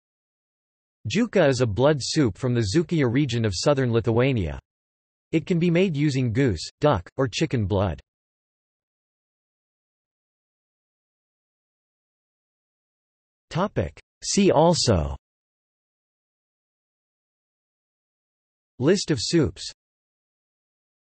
Juka (soup) | Wikipedia audio article
This is an audio version of the Wikipedia Article:
Juka (soup)

00:00:09 undefined

- Socrates

SUMMARY
Juka is a blood soup from the Dzūkija region of southern Lithuania. It can be made using goose, duck, or chicken blood.